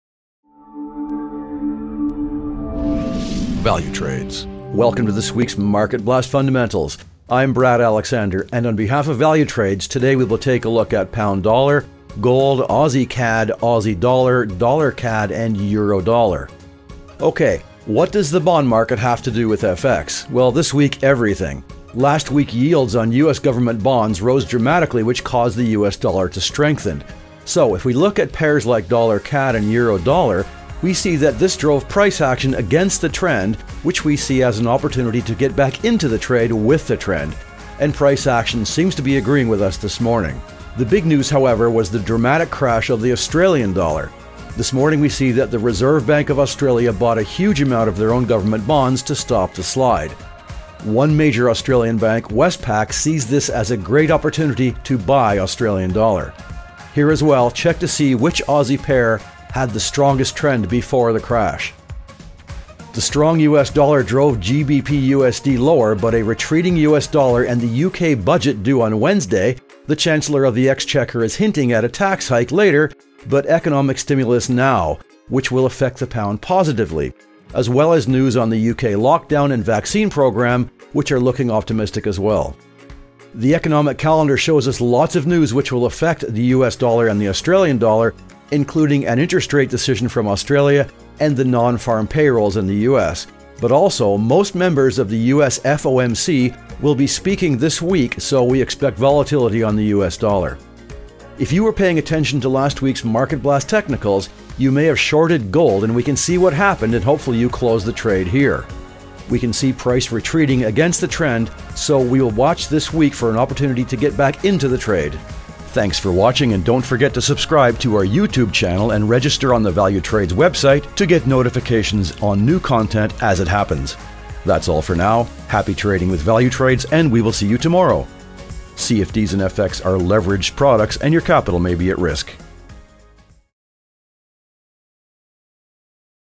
交易AUDUSD多头？ 一家大银行说："是的！"
欢迎来到本周的市场动态基础知识。

我是Brad Alexander，今天代表Valutrades，我们将看一下GBPUSD，黄金（XAUUSD），AUDCAD，AUDUSD，USDCAD和EURUSD。

好的。 债市和外汇有什么关系？

好吧，这周……将表示一切关联！

上周美国政府债券收益率大幅上升，导致美元走强。

因此，如果我们看一下美元加元和欧元兑美元等货币对，我们看到这推动了价格走势与趋势相反，我们认为这是一个随着趋势重回交易的机会，而今天上午价格走势似乎验证了我们的想法。

然而，最大的新闻是澳元的戏剧性暴跌。

今天上午我们看到，澳储行大量买入自己的政府债券，以阻止下滑。

澳大利亚一家大型银行--西太平洋银行--认为这是买入澳元的好机会。

这里也是，看看在暴跌前，哪个澳元对的走势最强。

强势美元带动英镑兑美元下跌，但随着美元的回落，周三将公布英国预算案，财政大臣暗示稍后将加税，但现在的经济刺激措施，将对英镑产生积极影响，以及英国封锁和疫苗计划的消息，看起来也很乐观。

经济日历显示，很多消息将影响美元和澳元，包括澳大利亚的利率决定和美国的非农就业，但同时，大多数美国FOMC成员将在本周发言，因此我们预计美元将出现波动。

如果您关注上周的市场动态技术知识，您可能已经做空了黄金，我们可以看到发生了什么，并希望您已完成交易。

我们可以看到价格回落，逆势而上，所以本周我们将观察是否有机会重新进入交易。

今天就到此结束。 祝您与Valutrades交易愉快，我们明天见。

差价合约和外汇是杠杆产品，您的资金可能面临风险。